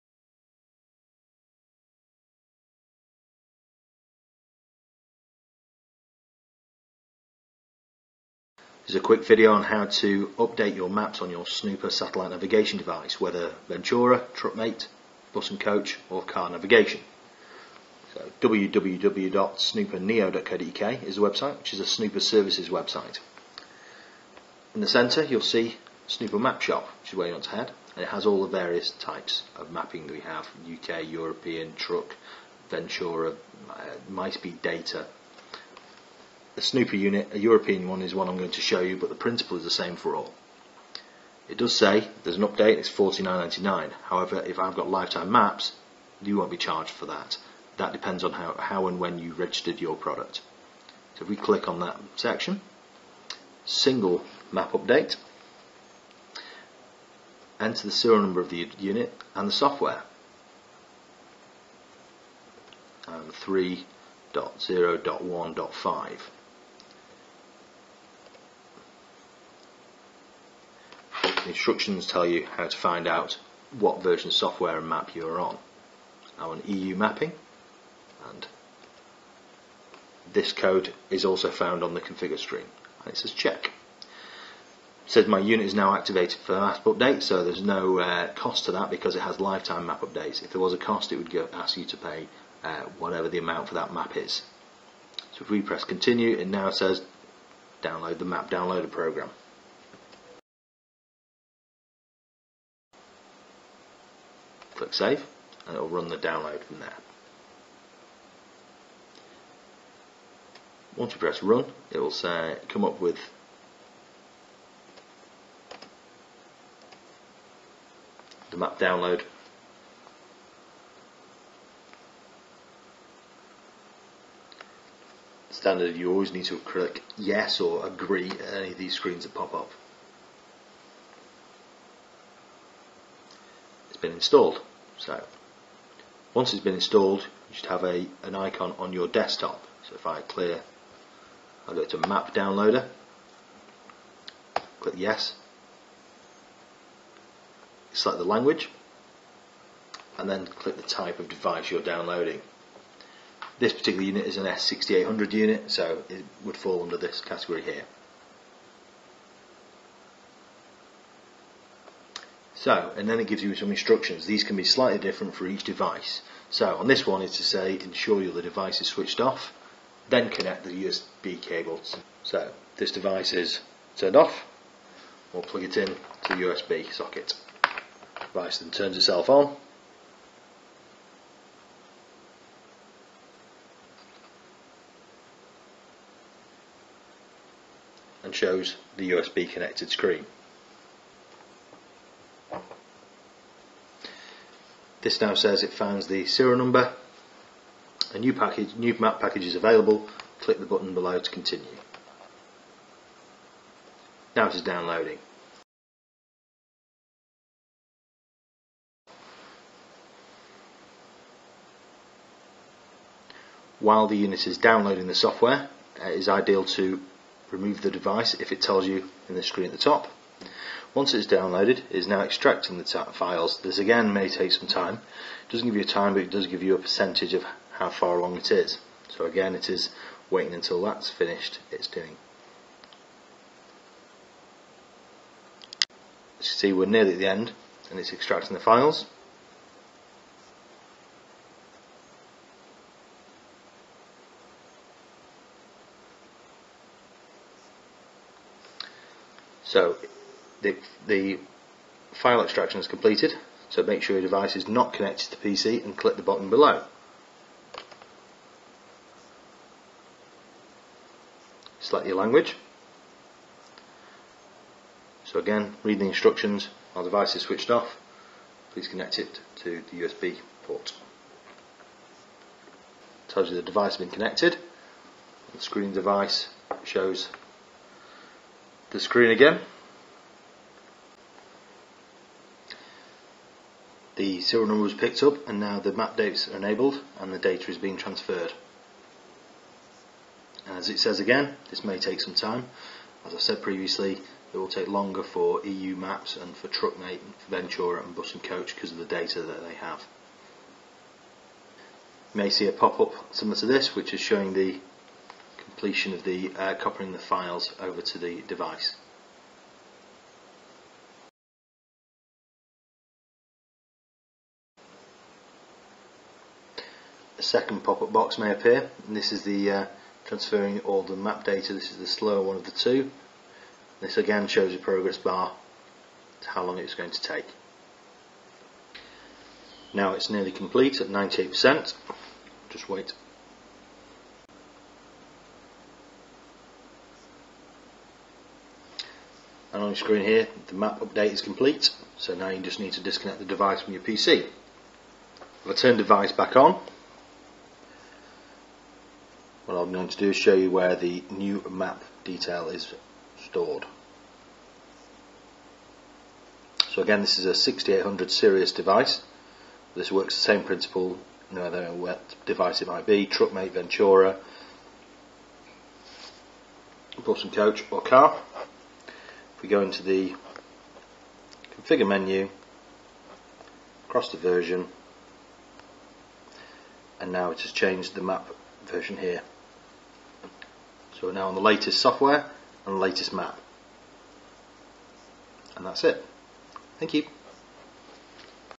Downloading Maps to Snooper Sat Nav Truckmate, Ventura, Syrius, Bus & Coach
How to download your Snooper Satellite Navigation unit, update mapping software.